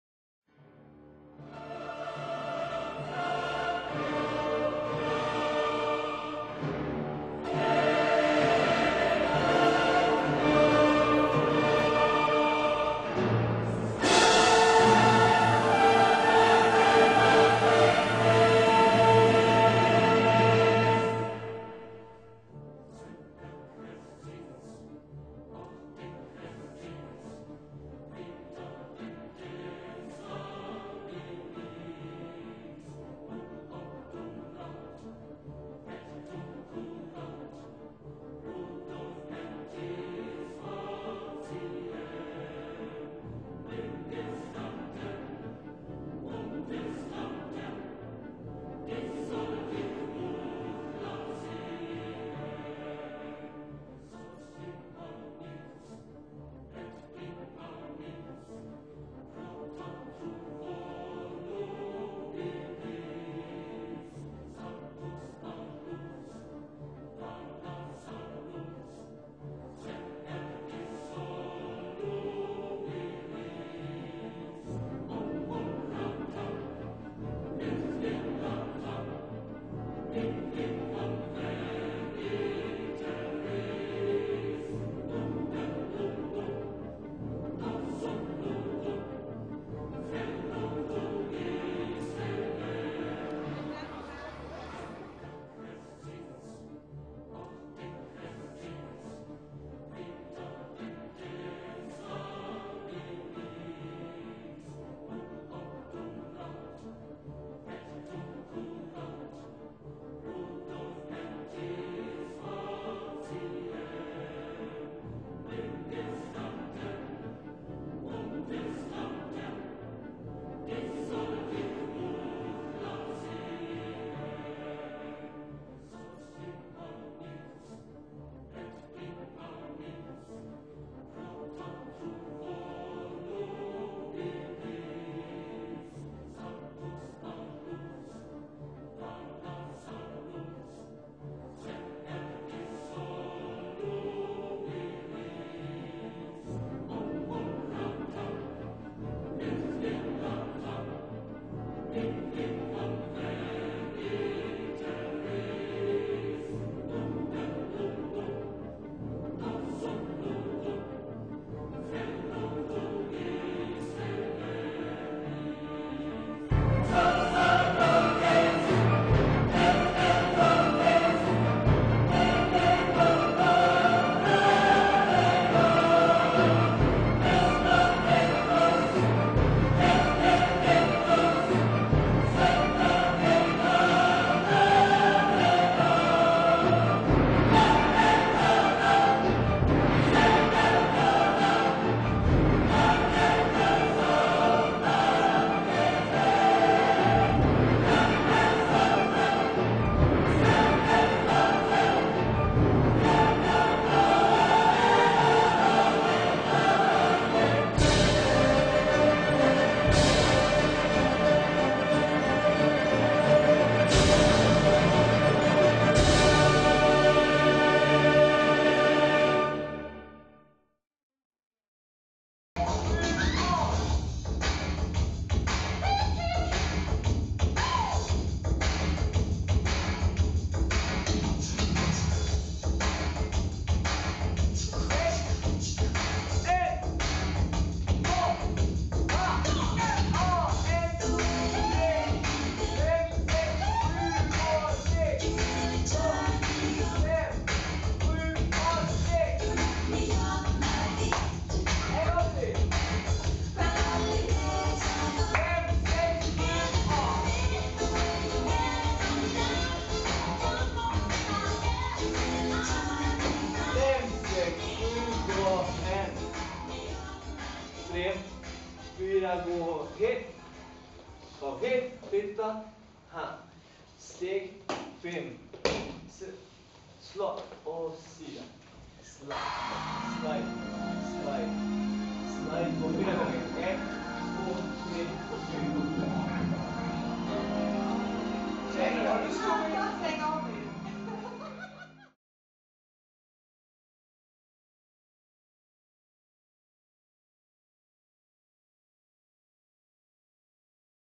Mazen's Dance Video Collection -2012
Mazen Do Brazil, is a collection of dance videos during the coarse of two years. Dancing experience from my Jönköping Dance Classes and my travels around the world, Italy, Spain, Brazil, Africa, India and more. There will be more videos from all the experiences that i will upload soon.

A dance tribute to all dance lovers in the world.